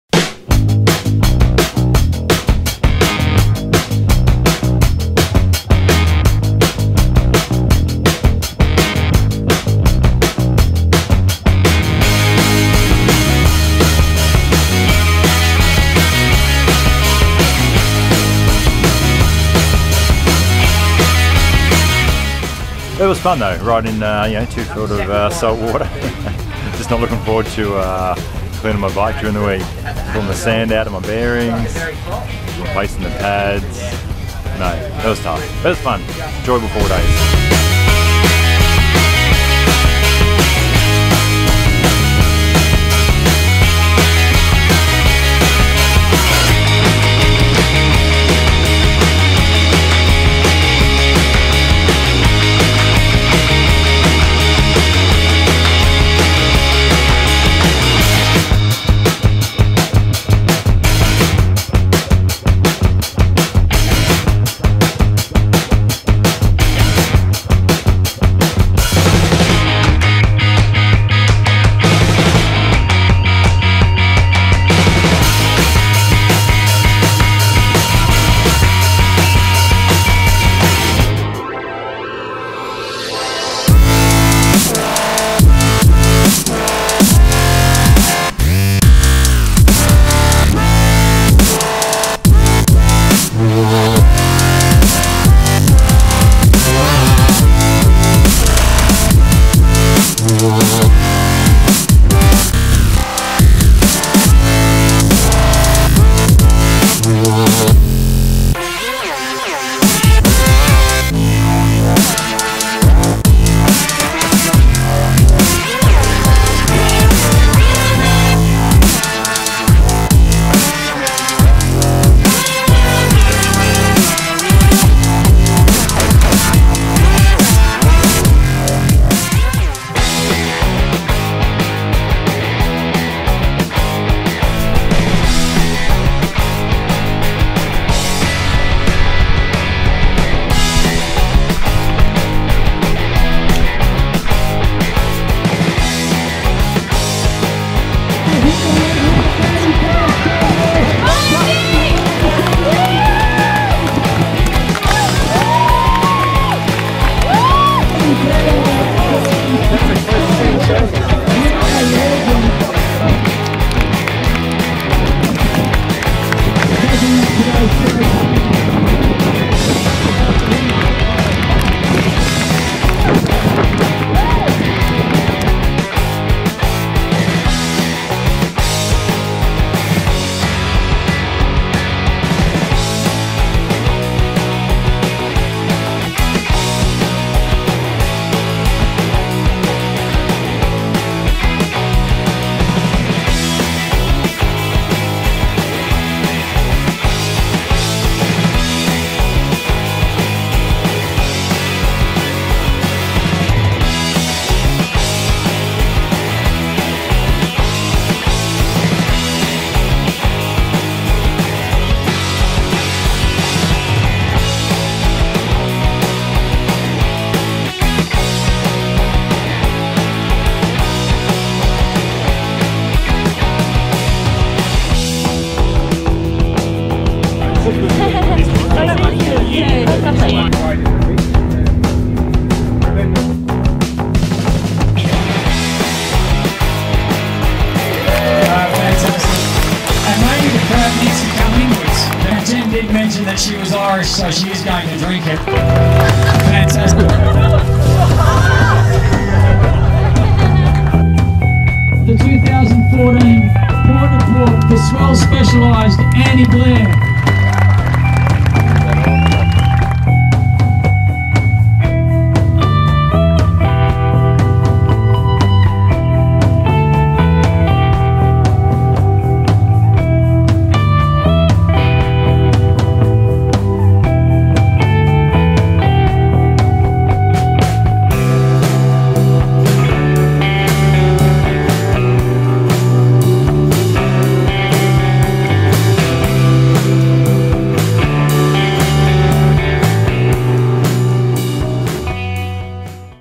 Port to POrt - Day 4 - Cams Wharf to Nobbys
The final dramatic stage of the Port to Port MTB 2014 4 day stage race. This stage takes us from Cams Wharf to Nobbys Beach via Swansea, Blacksmiths Beach, Fernleigh Track, through Glen Rock Recreation Reserve then along the picturesque coastline of Newcastle via Merewether Beach and Bar Beach then finishing at the iconic Nobbys Beach.